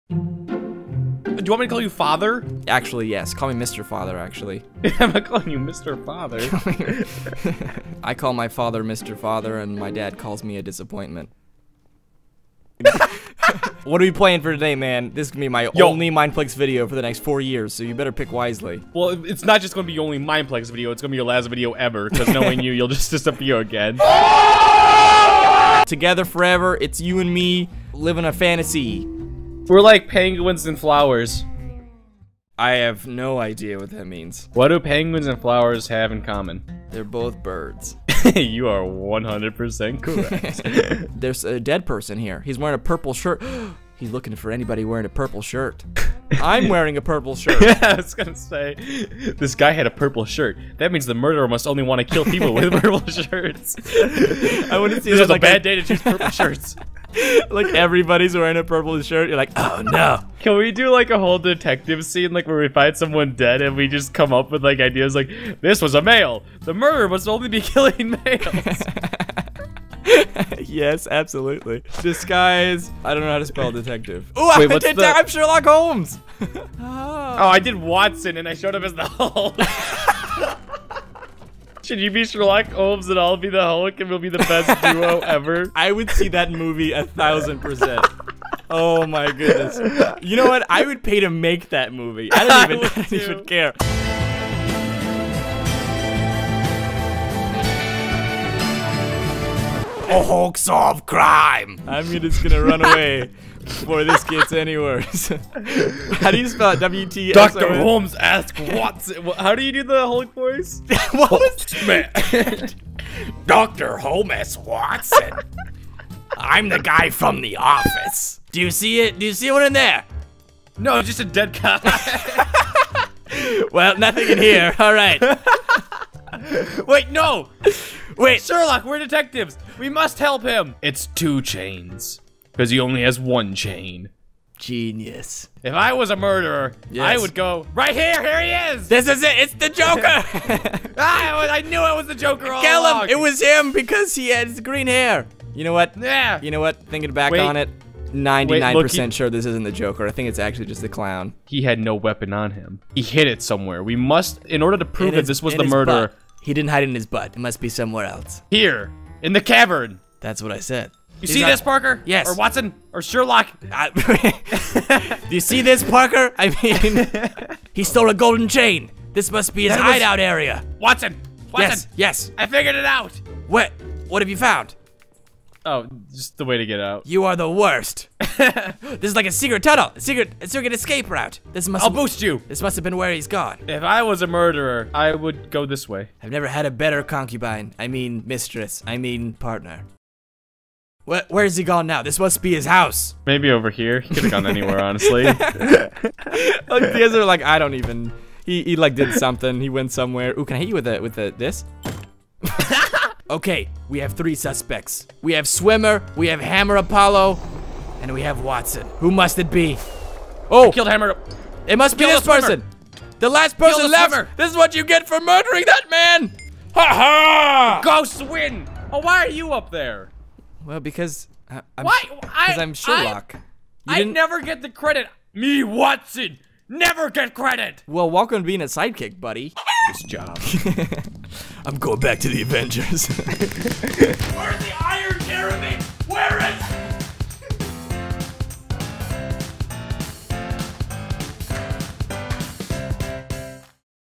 TERRIBLE DETECTIVES!
Today, Parker and HBomb try out their life long dream of becoming a detective duo.

SEND ME STUFF FOR MY WALL! :D
-3950 Laurel Cyn Blvd